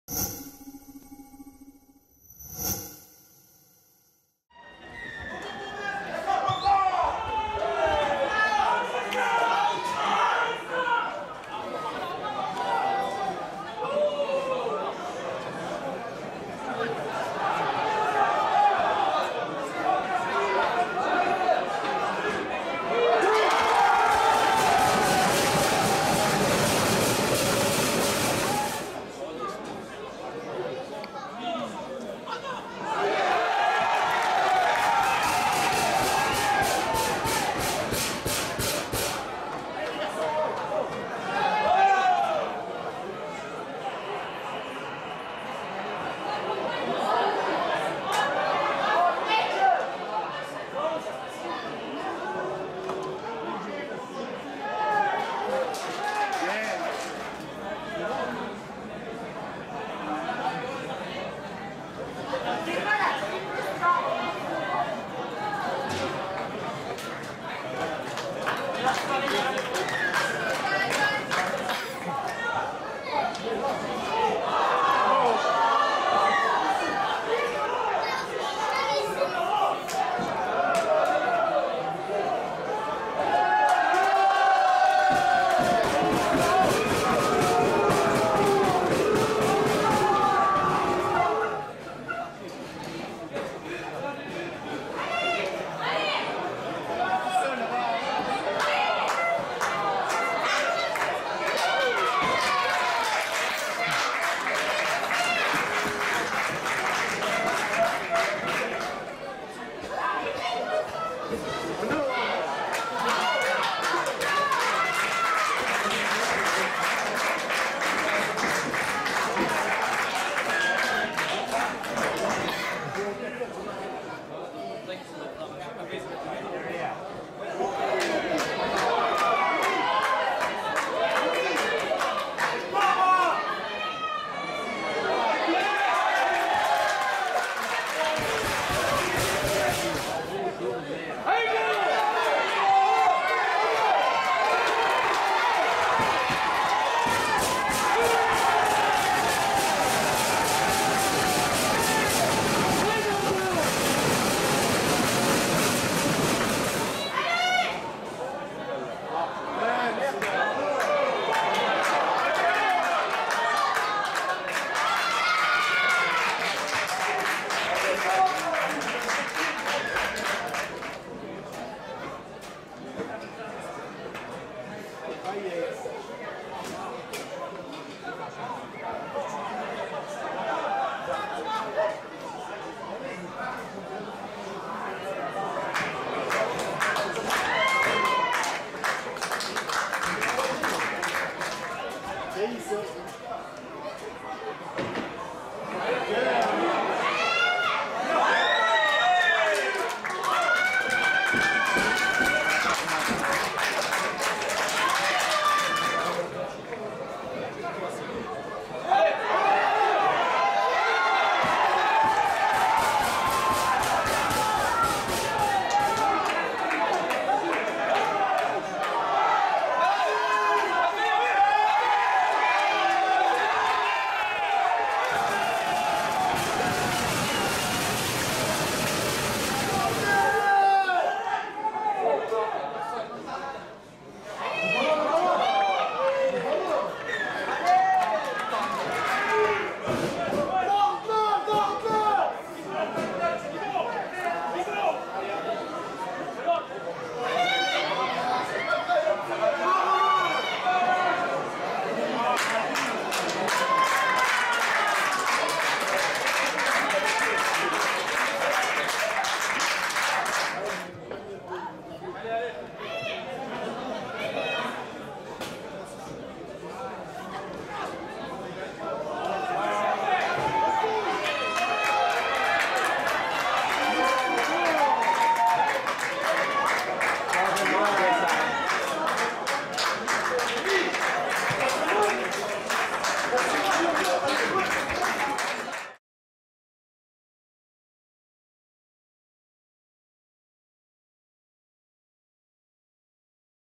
Résumé Carpentras vs St Estève - XIII Catalan (Coupe)
Les plus belles actions et tous les essais du 1/4 de finale de Coupe Lord Derby entre St Estève - XIII Catalan et Carpentras.